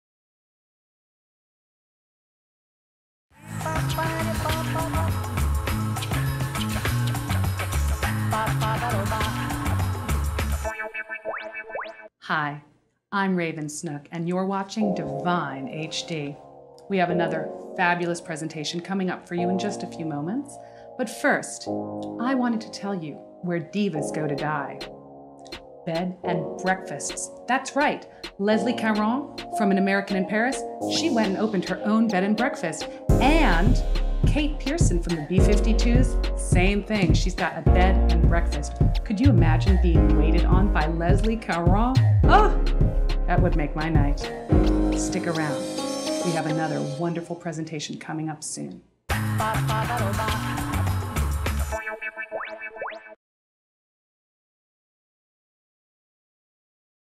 Raven Snook on Movie Bed & Breakfasts, Divine HD 2004
In 2004 Raven Snook was an on-air host at DIVINE HD, an NYC cable channel showing a library of MGM films with a gay appeal.   Raven joined a team of other NYC area artists, writers and drag queens who would introduce the films.   Sometimes Raven would tell a brief story between films, like this one.